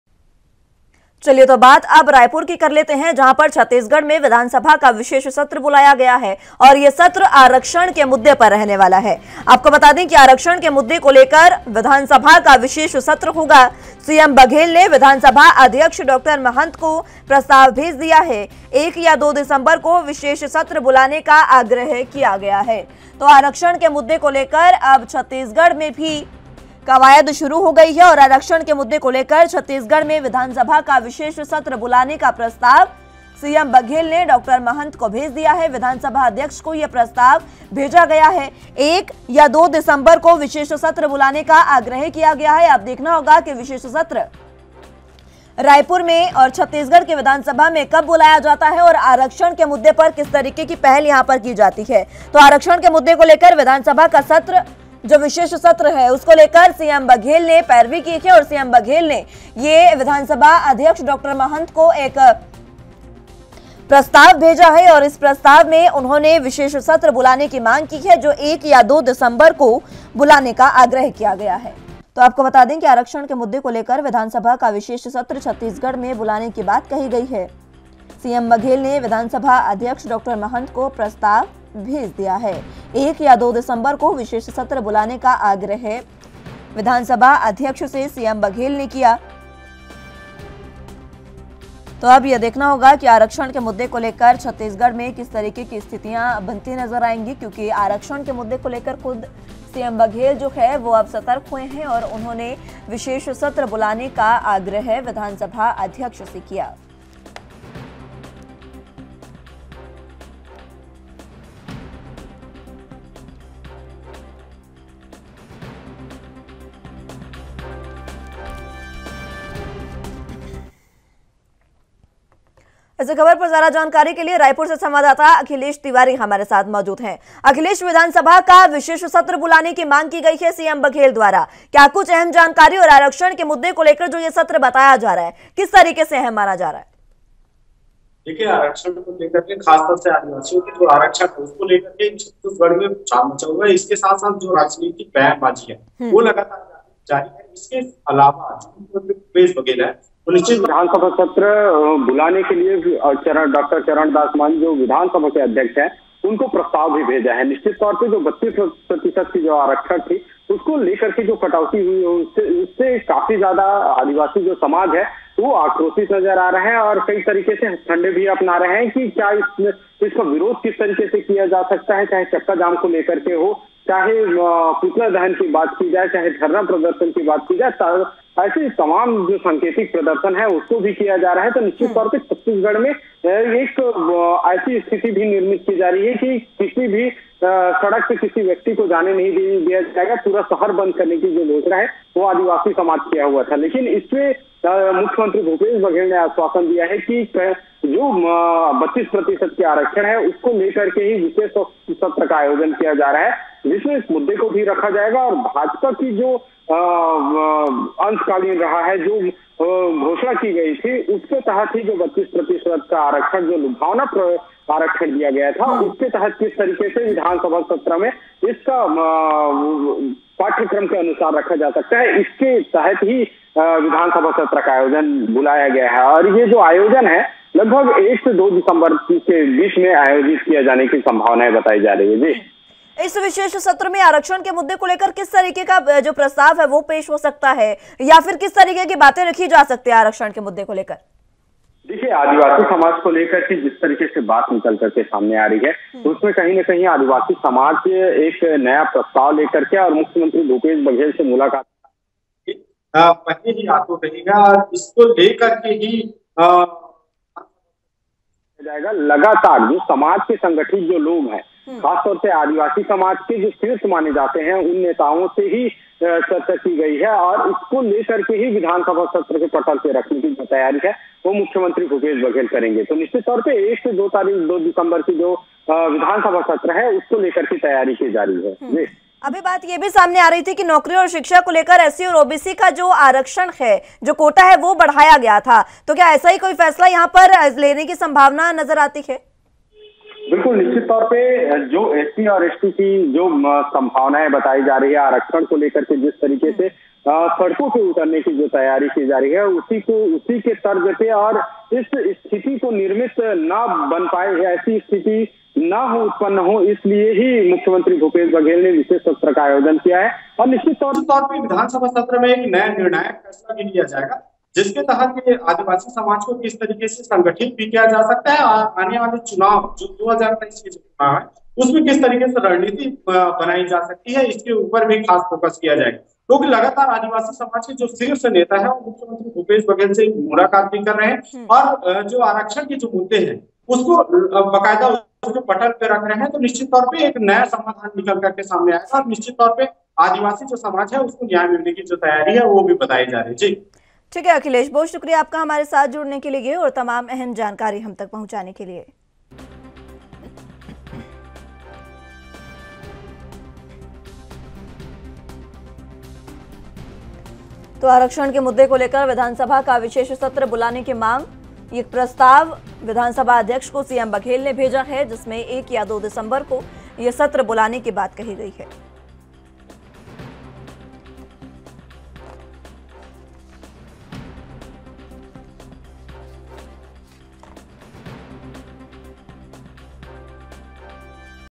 आरक्षण के मुद्दे को लेकर होगा विधानसभा का विशेष सत्र | ANB NEWS
आरक्षण के मुद्दे को लेकर होगा विधानसभा का विशेष सत्र
सीएम बघेल ने विधानसभा अध्यक्ष डॉ. महंत को भेजा प्रस्ताव
एक या दो दिसंबर को विशेष सत्र बुलाने का आग्रह